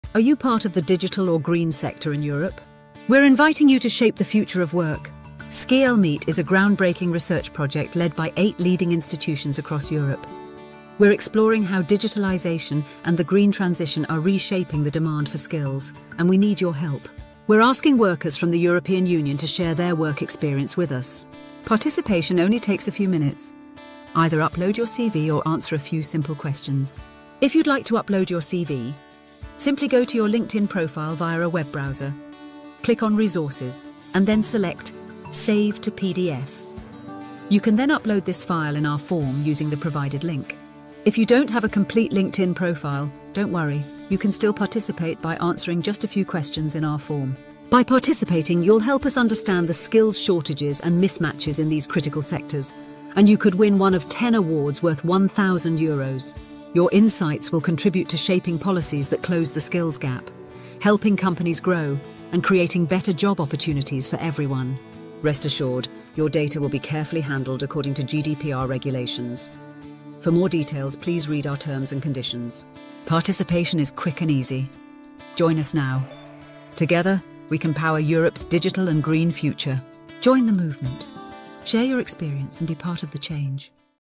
SkilMeet (English)
SkiLMeet is a groundbreaking research project led by eight leading institutions across Europe. We’re exploring how digitalisation and the green transition are reshaping the demand for skills, and we need your help. Are you working in the green or digital sector? Then we invite you to become part of SkiLMeeT by sharing your recent resume with us. By participating you can make a chance of winning a 1000€ prize.